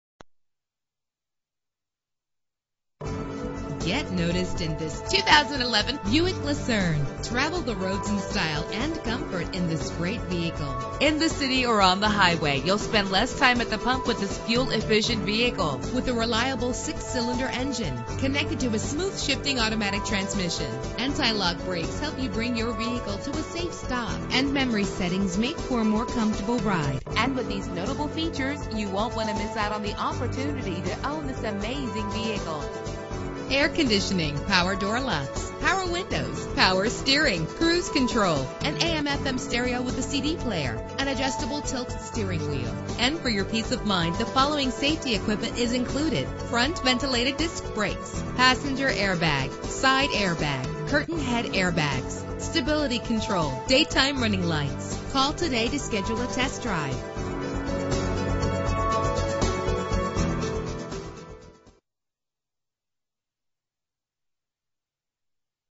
2011 Buick Lucerne - Worcester MA
Year: 2011
Make: Buick
Model: Lucerne
Trim: CXL
Engine: 3.9 liter 6 cylinder 12 valve
Transmission: 4-Speed Automatic
Color: Light Bronze Metallic
Mileage:
Address:
520 Park Avenue